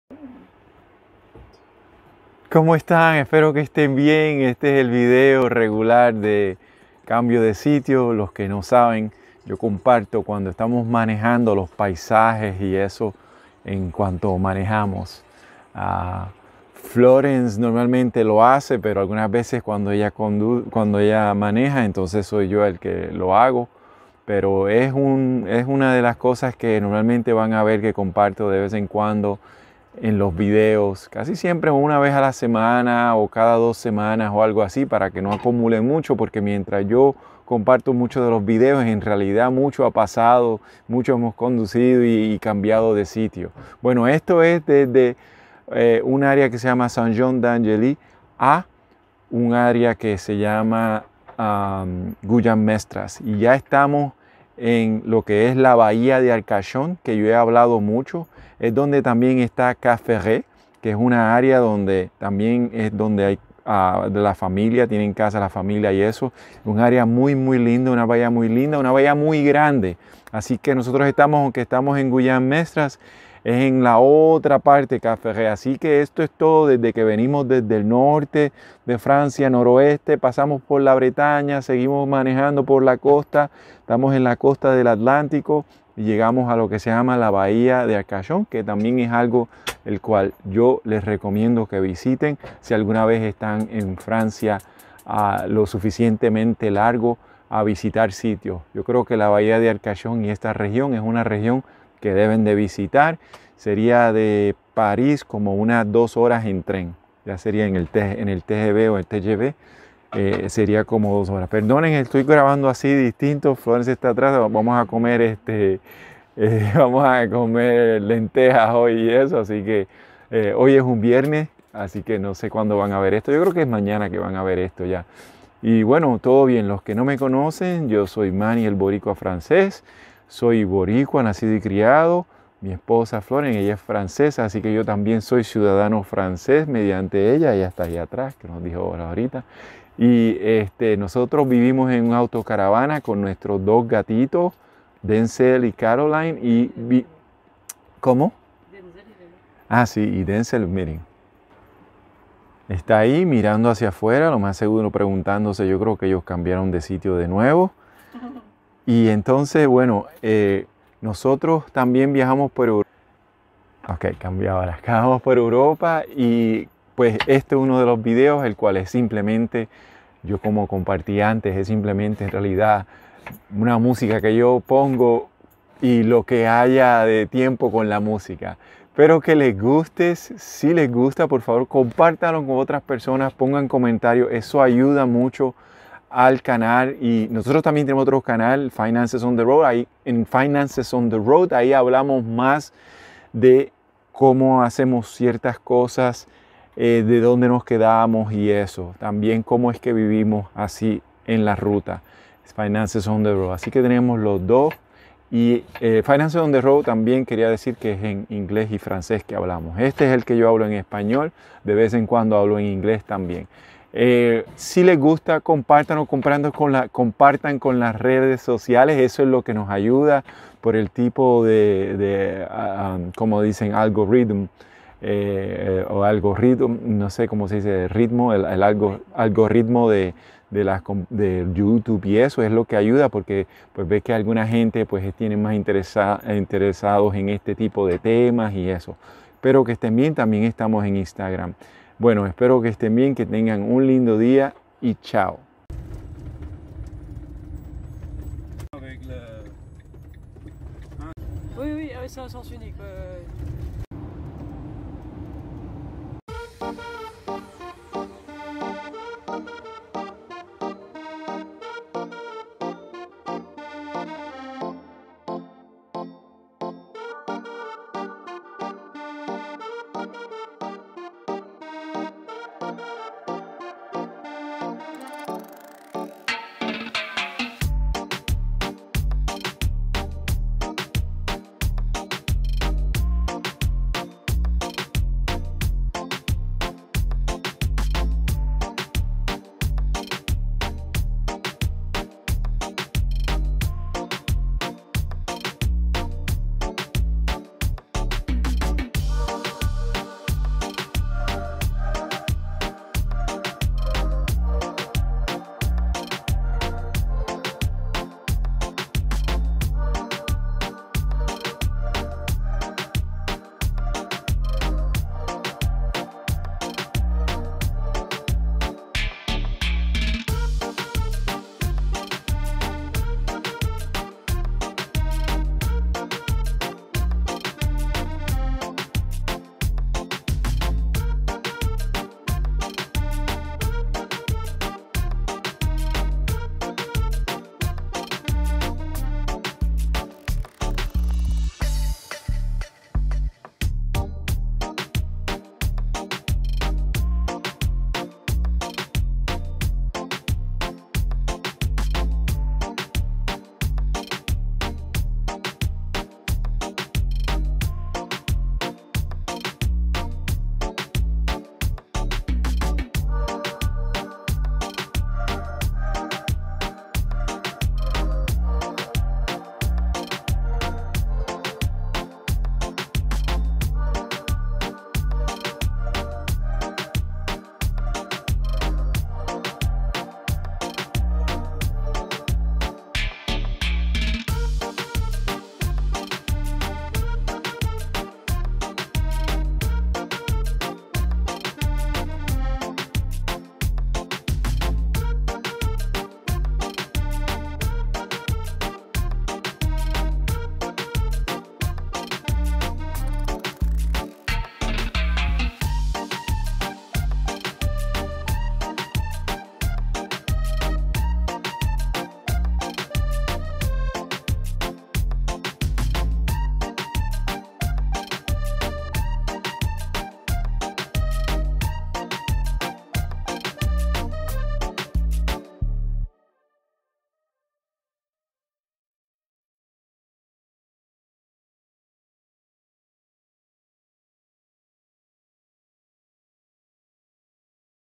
Cambio de sitio. De Saint Jean D'Angely a Gujan-Mestras
Nuestro tipico video de paisajes mientras manejamos. Fuimos de el pueblo de Saint Jean D'Angely al pueblo de  Gujan-Mestras.  le recomiendo visitar ambos.🥰✌🏾
Our typical video of landscapes while we drive.  We went from the village of Saint Jean D'Angely to the village of Gujan-Mestras.  I recommend visiting both .🥰✌🏾